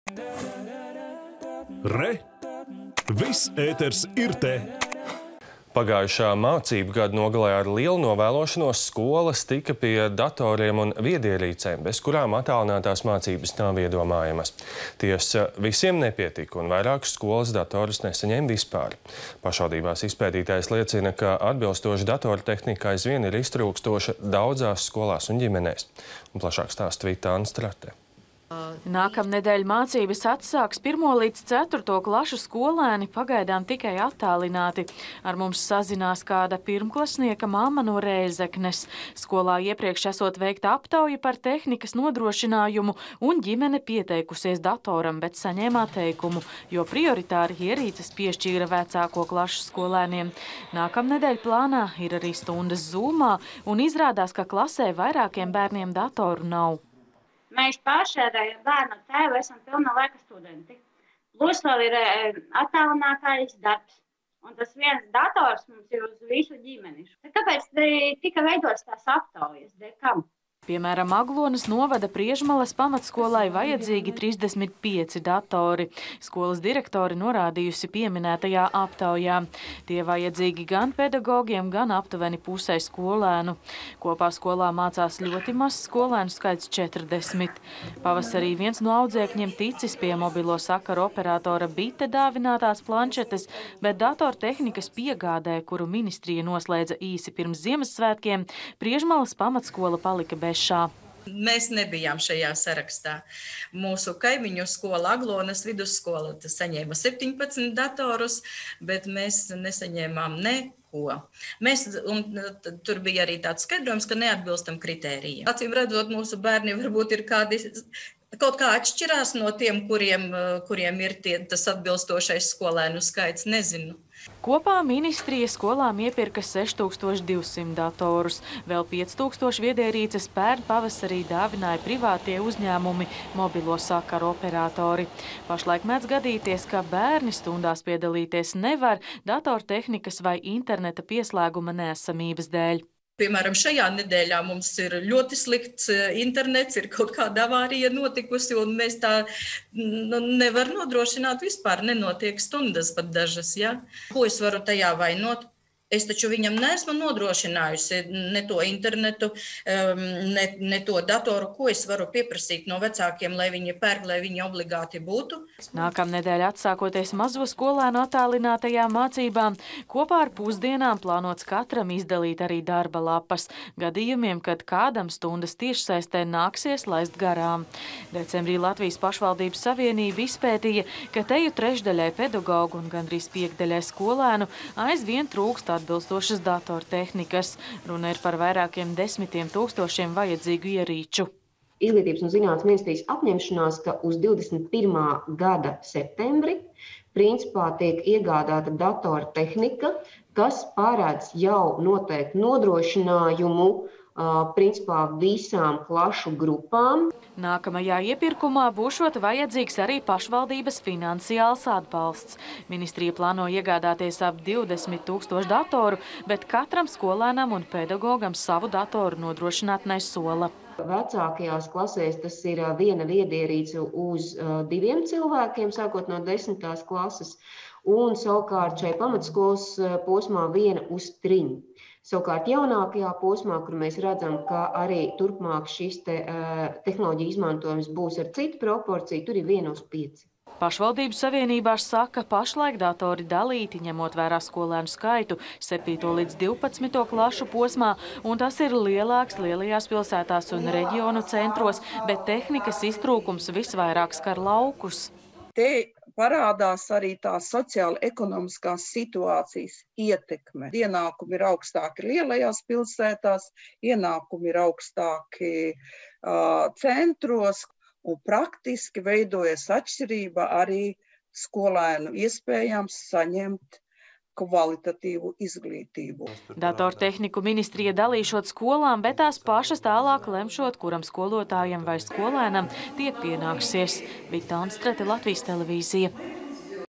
Datoru katram skolēnam valsts nopirkt neplāno
Pagājušā mācību gada nogalē ar lielu novēlošanos skolas tika pie datoriem un viedierīcēm, bez kurām attālinātās mācības nav iedomājamas. Tiesa, visiem nepietika un vairākas skolas datorus nesaņēma vispār. Pašvaldībās izpētītais liecina, ka atbilstoša datortehnika aizvien ir iztrūkstoša daudzās skolās un ģimenēs. Stāsta Vita Anstrate.